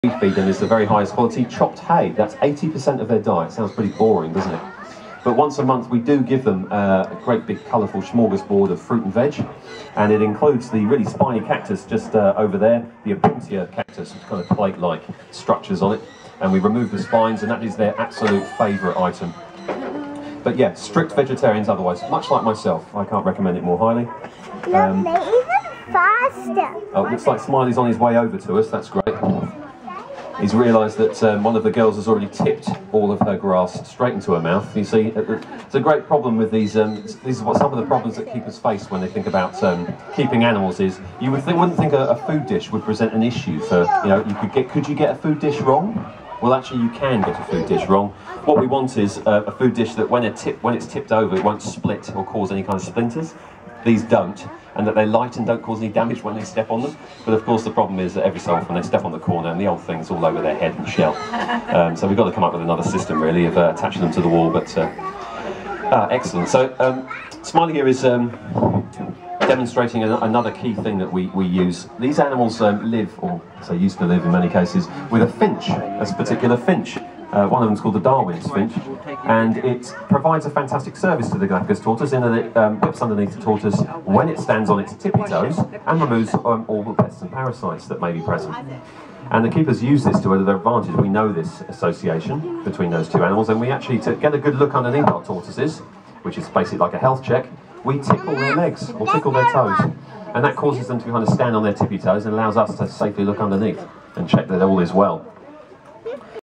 2017 Auckland Zoo trip with Olivia (Galapagos Tortoises)
We were delighted to arrive at the Galapagos Tortoises' flash new enclosure to discover their keeper was giving a talk.  As Olivia had been fascinated with the tortoise creche in the Zoo's Pridelands section, we hung around to discover more about these incredibly rare creatures, as these are the only four in New Zealand.